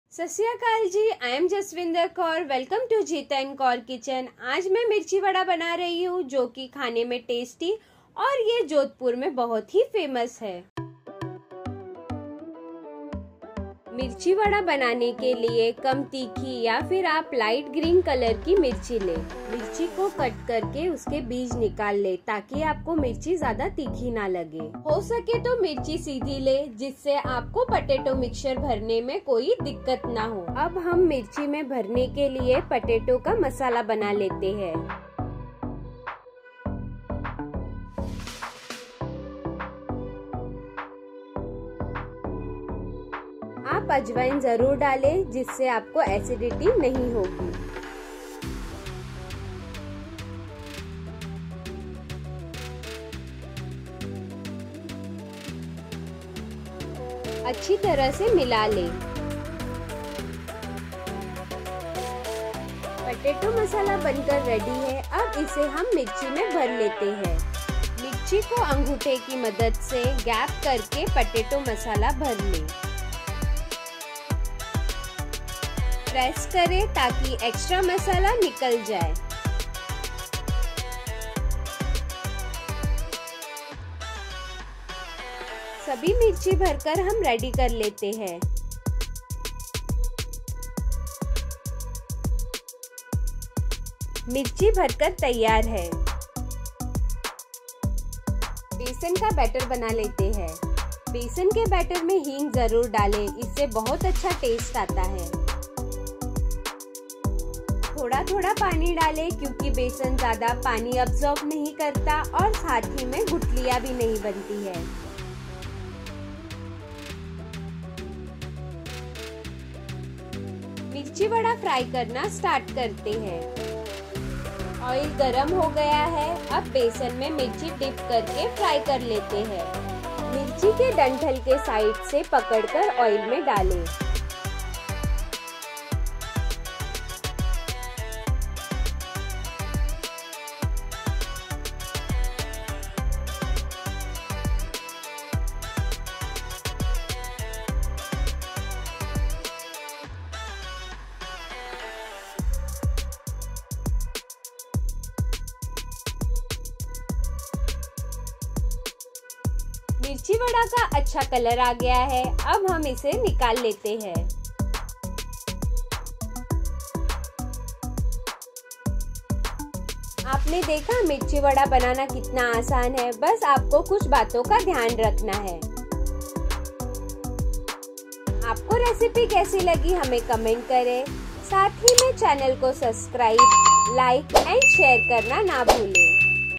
Mirchi Vada | जोधपुर का मिर्ची वडा | mirchi bada | mirchi vada recipe | how to make mirchi bada
Jodhpuri Mirchi Vada is an Indian snacks stuffed with potatoes.
It is battered of gram flour.
Boiled potatoes accompanied by Chilli Powder, Salt, Aamchur powder and spices. Potatoes mixtures stuffed in chilies, dipped in the gram flour battered and fried. This tastes yummy with tamarind chutney/ Tomato ketchup/ mint chutney...

Ingredients:
Mirchi vada
• Less spicy chilli 250gm - कम तीखी मिर्च

Potato masala
• Boiled potato 6 (medium) -  उबले आलू
•Turmeric ¼ tsp - हल्दी
• Garam masala ½ tbsp - गरम मसाला
• Kashmiri red chilli 1 tbsp - कश्मीरी लाल मिर्च
• Aamchur powder ½ tbsp - अमचूर पाउडर
• Carom seed ¼ tsp - अजवायन
• Salt to taste - नमक स्वादअनुसार
• Green chilli (chopped) 1 nos. - हरी मिर्च
• Coriander – धनिया

Besan batter
• Gram flour 2 cup - बेसन
• Hing ¼ tsp - हिंग
• Baking soda 1/4 tsp -  बेकिंग सोडा
• Water – पानी

• Oil for fry - तलने के लिए तेल